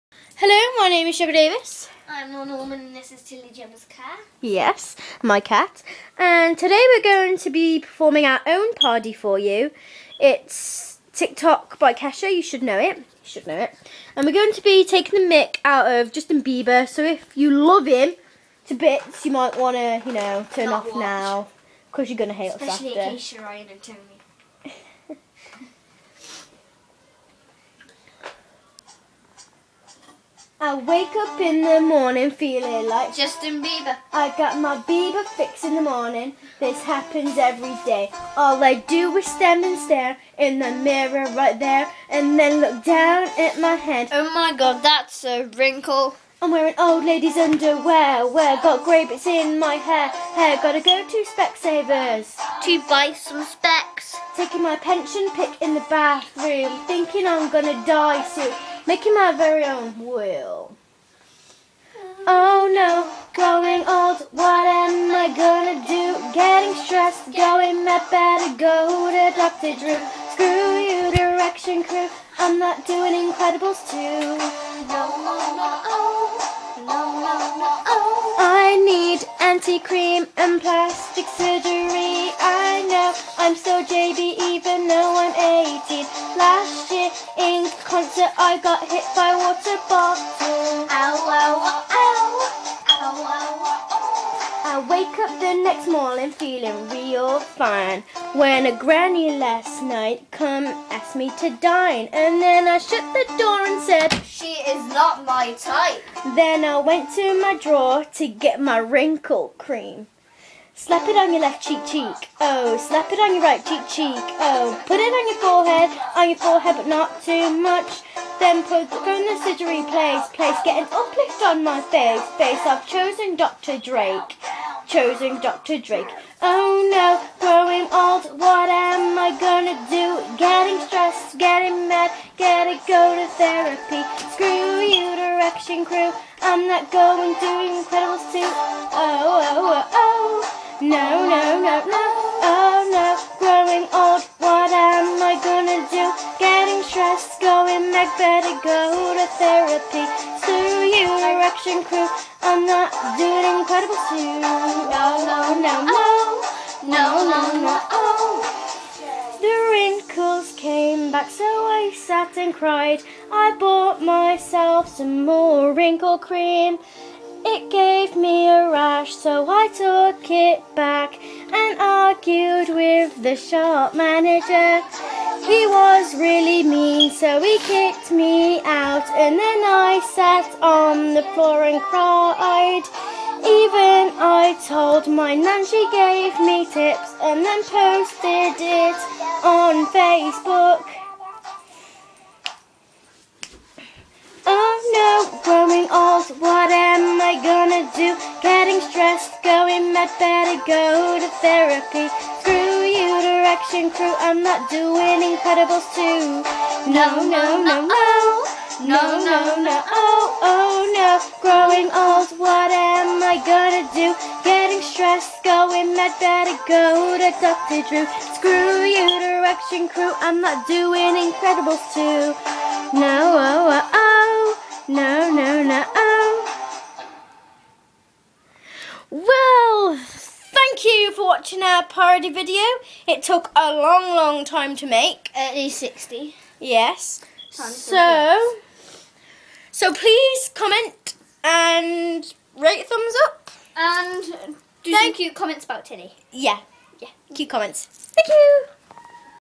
tik tok spoof gemma and laurna
We wanted to make own vidio of tik tik so here it is        by the way i love thecmputernerd01  vidios